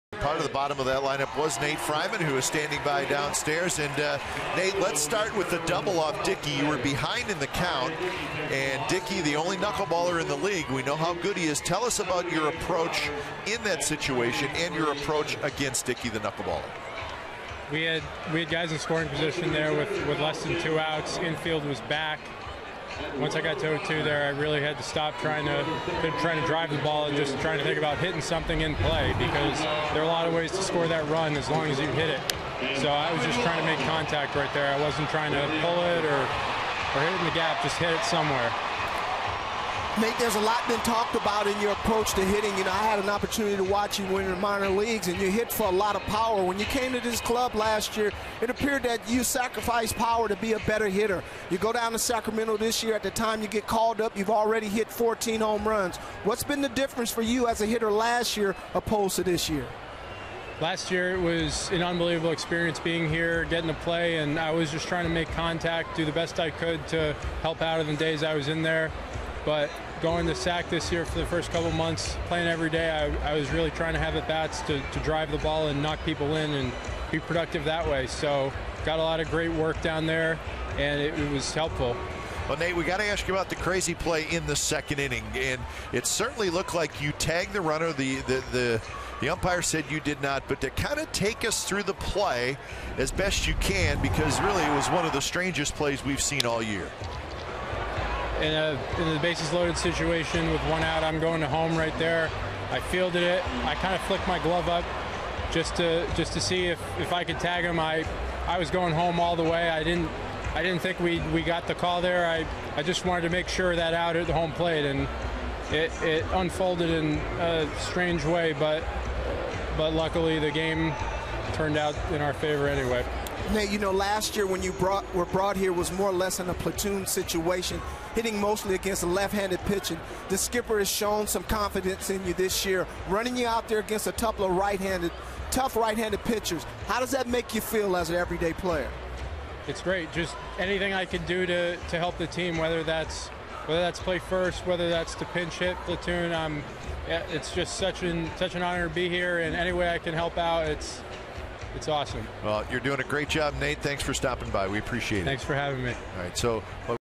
TOR@OAK: Freiman discusses his RBI double, A's win
7/3/14: Nate Freiman talks about his RBI double in the 2nd inning, the disputed call in the 2nd inning and his team coming home and winning